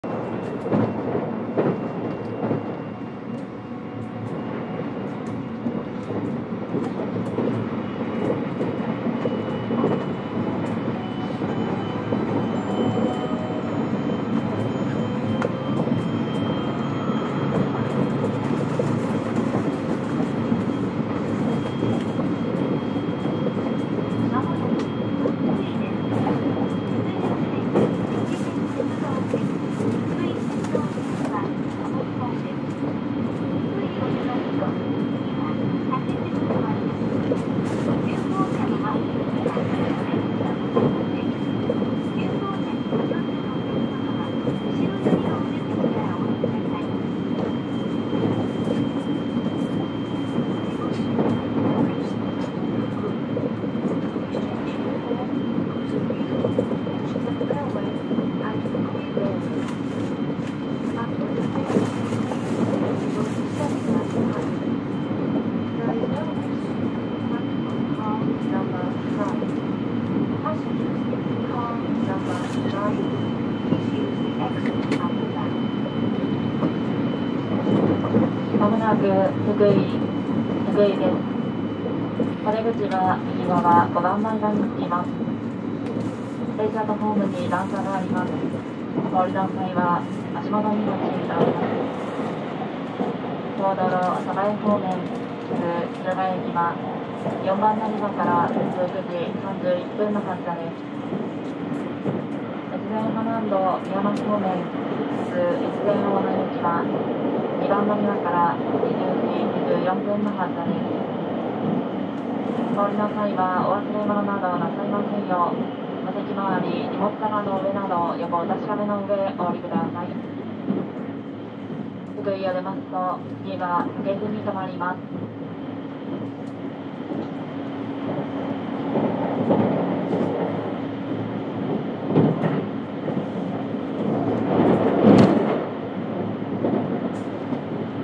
サンダーバード44号 福井駅到着前車内放送 ＊2023年2月3日録音
サンダーバード44号福井駅到着前の車内放送です。